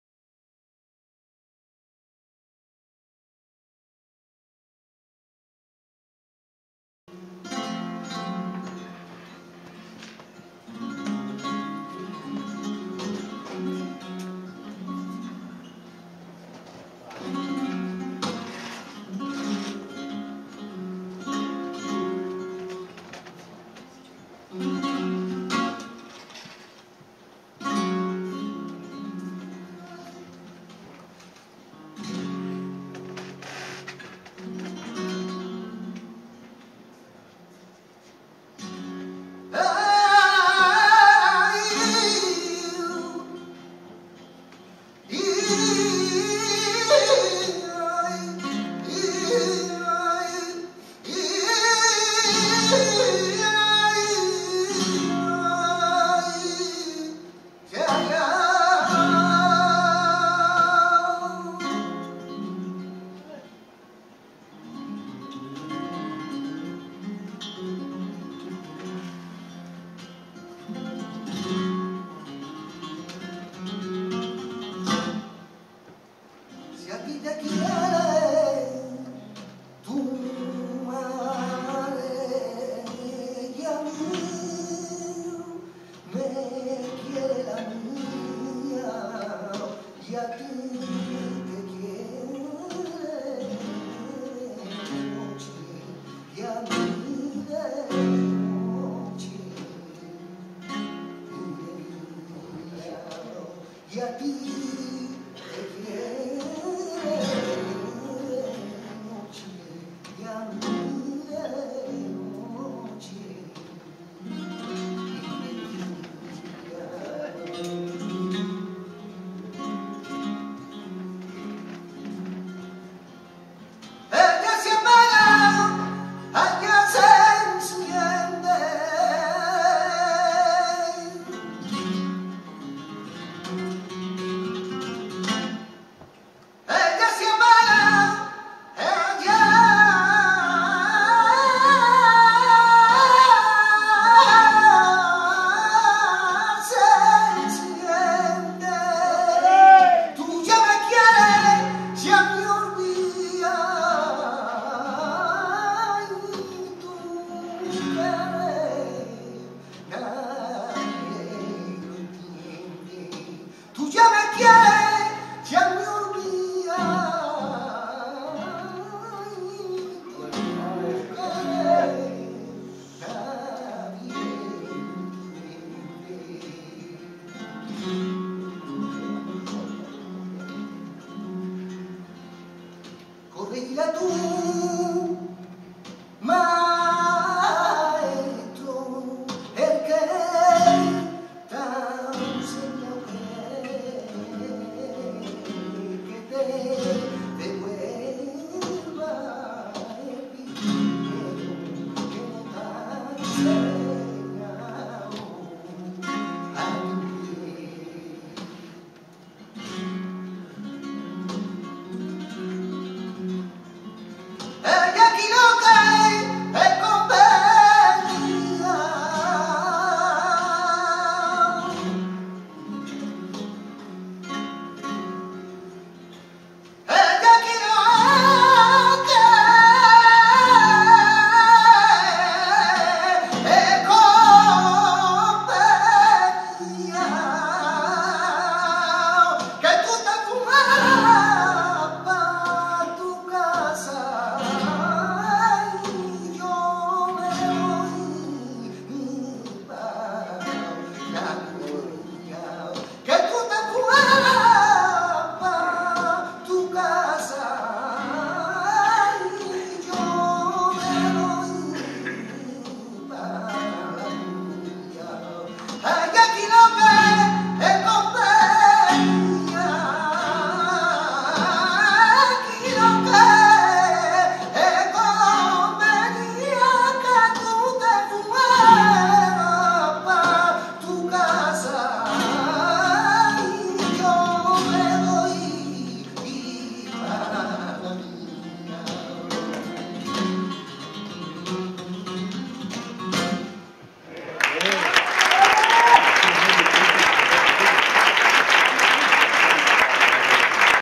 Sergio El Colorao, XXII Concurso Nacional de Arte Flamenco, Córdoba
Sergio El Colorao, Rafael Santiago Habichuela
XXII Concurso Nacional de Arte Flamenco, Córdoba
Teatro Góngora